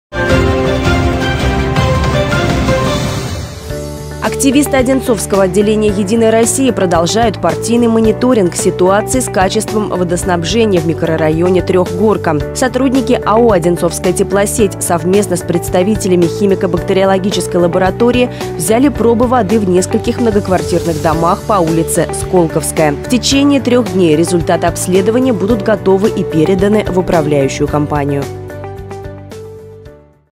Партийцы держат на контроле ситуацию с водоснабжением Трёхгорки
Активисты одинцовского отделения «Единой России» продолжают партийный мониторинг ситуации с качеством водоснабжения в микрорайоне Трехгорка.